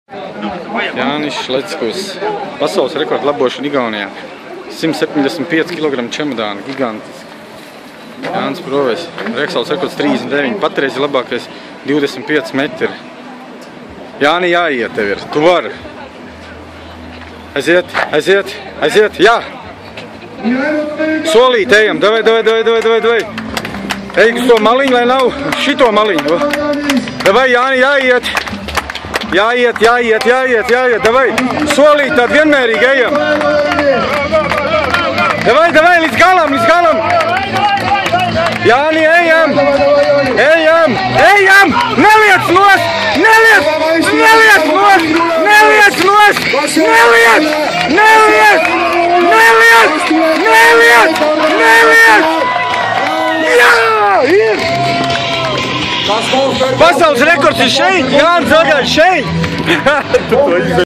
NEW WORLD RECORD WITH 175 KG FARMER'S WALK - 40,6 METER'S - JĀNIS ŠLECKUS
Hi beat 13 years old record ,witch made in 2001 Olympic Teams , Hungary- 175 Kg Farmers walk – 39 meter Wout Zijlstra ,Holland !